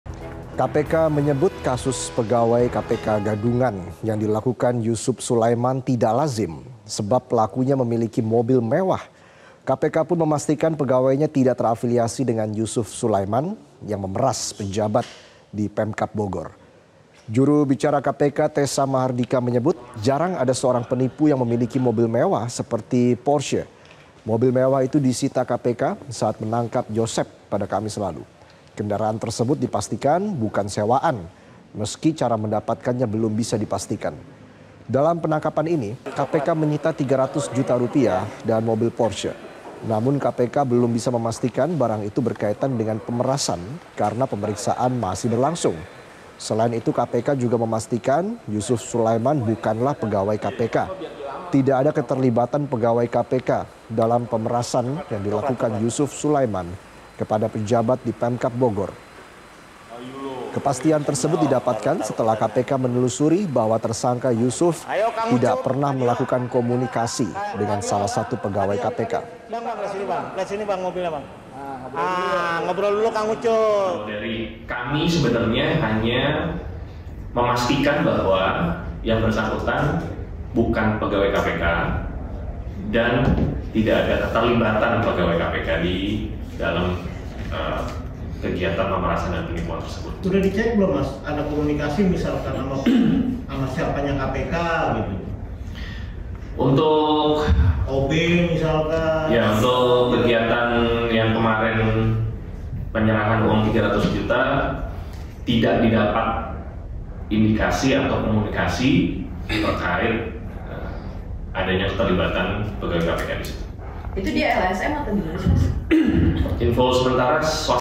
Pegawai KPK Gadungan Peras Pejabat Pemkab Bogor
MetroTV, Polisi menetapkan Pegawai KPK gadungan Yusuf Sulaiman yang telah melakukan pemerasan kepada ASN di Pemkab Bogor.

Follow juga sosmed kami untuk mendapatkan update informasi terkini!kpk